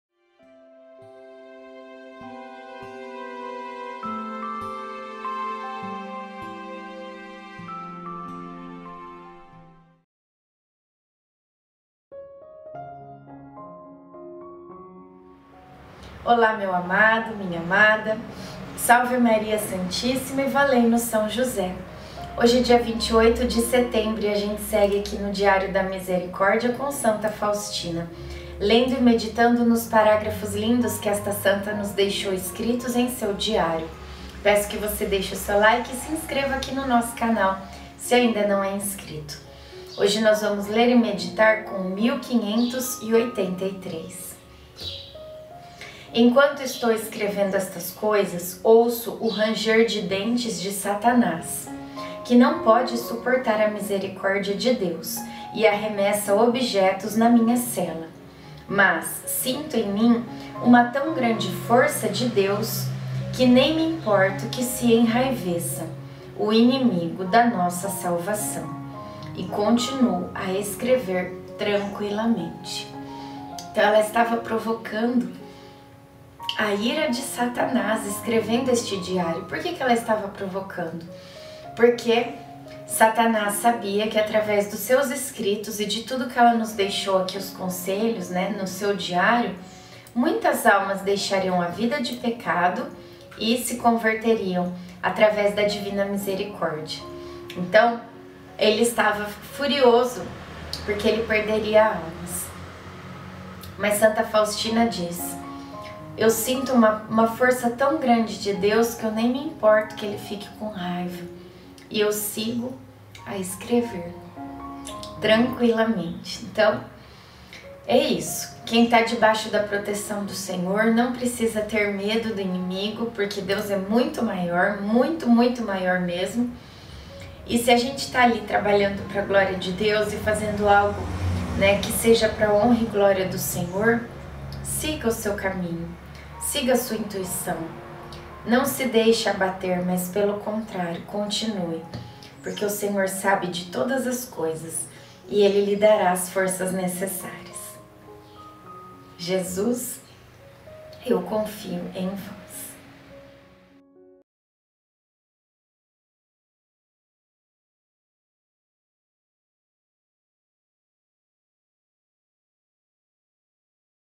DIÁRIO DA MISERICÓRDIA COM SANTA FAUSTINA 28/09/24
1583Enquanto estou escrevendo estas coisas, ouço o ranger de dentes de
satanás, que não pode suportar a misericórdia de Deus e arremessa
objetos na minha cela. Mas sinto em mim uma tão grande força de
Deus que nem me importo que se enraiveça (152) o inimigo da
nossa salvação, e continuo a escrever tranquilamente.

Deus abençoe!